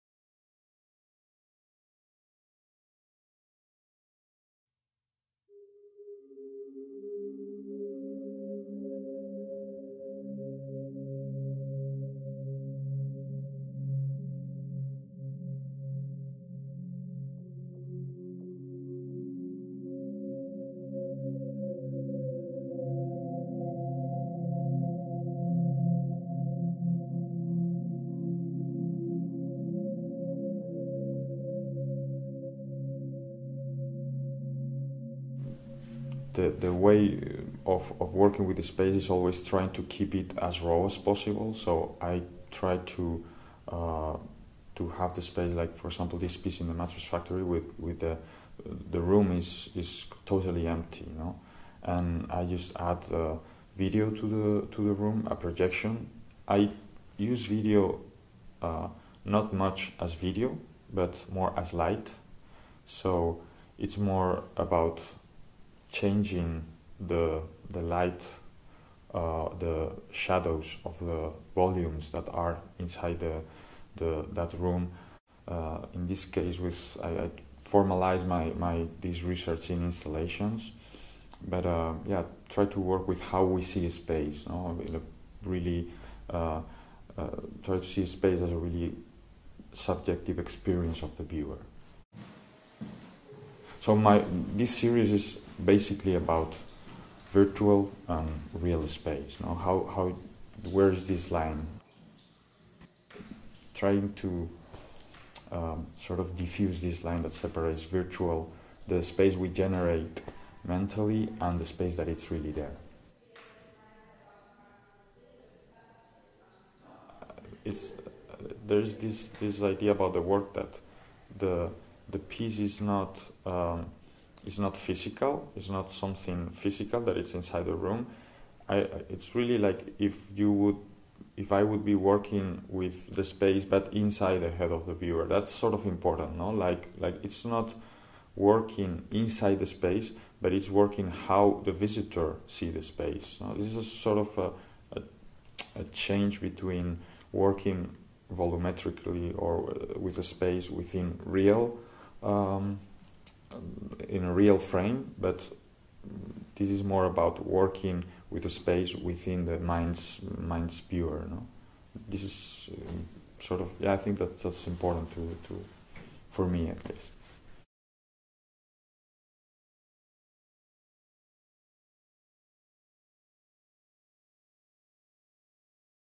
Pablo Valbuena - Para-Site [mattress factory]
PABLO VALBUENA
Para-Site [mattress factory], 2011
video projection on architecture

Pablo Valbuena (Madrid, 1978) develops artistic projects and research focused on space, time and perception.

Some key elements of the construction of his work are the overlaping of physical and virtual space, the generation of mental spaces by the viewer, the dissolution of the limit between real and perceived, the unity of time and space, and the use of light and projection as the prime matter of his work.

He has presented his work recently at the Museum of the Moving Image (New York), 5th International Seoul Biennale (Seoul, Korea), OK Center for Contemporary Art (Austria), Laboral Centro de Arte (Spain), Netherlands Media Arts Institute (Amsterdam), BankArt (Japan), Matadero Madrid (Spain), Museum of Contemporary Art Taipei (Taiwan), gallery Max Estrella in Madrid and others. He has also presented public art projects in Singapore, Canada, France, Belgium, the Netherlands, USA, Brazil and Spain.

His large site-specific interventions in the public space have been seen in The Hague City Hall (the Netherlands), Museo Universitario de Arte Contemporáneo (Mexico City), Plaza de las Letras-Medialab Prado (Madrid), Murcia City Hall (Spain) and Vooruit-Ghent University (Belgium).